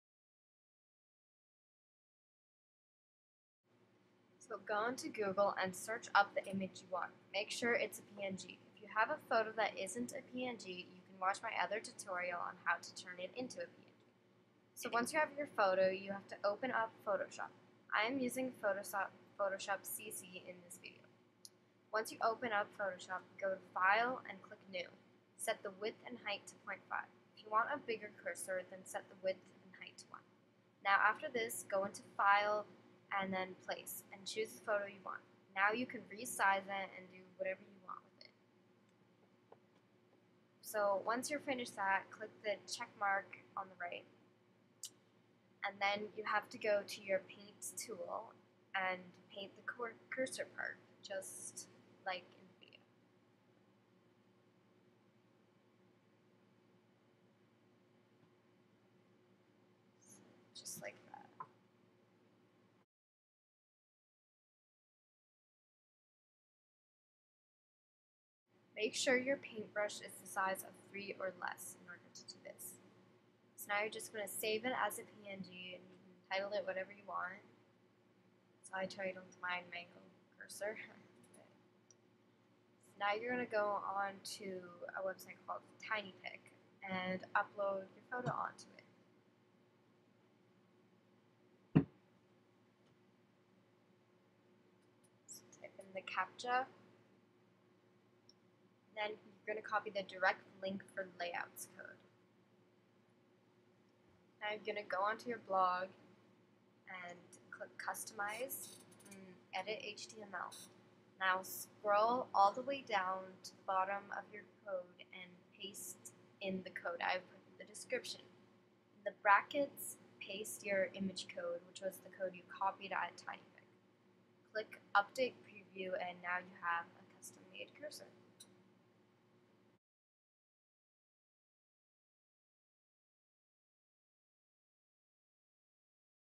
How to make a cursor for tumblr with Photoshop CC
How to make a custom image cursor for TUMBLR with Photoshop CC.